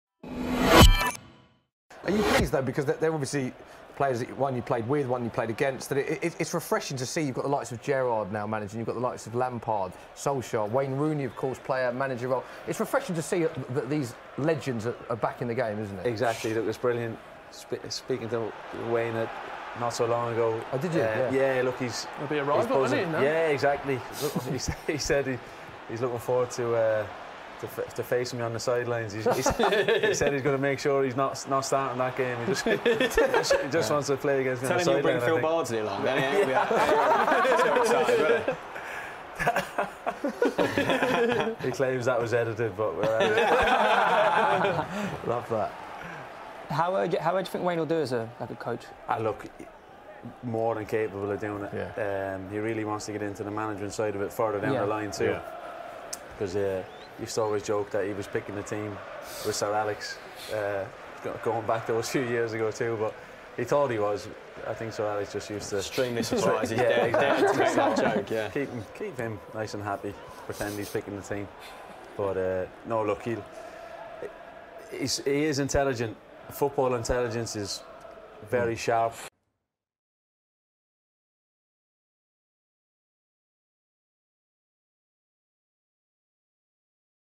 John O'Shea Reacts To Wayne Rooney Becoming Player-Manager At Derby
Former teammate John O'Shea on Wayne Rooney's new role at Derby County.

Sky Bet believe that betting should never get in the way of sport. Please gamble responsibly. 18+ only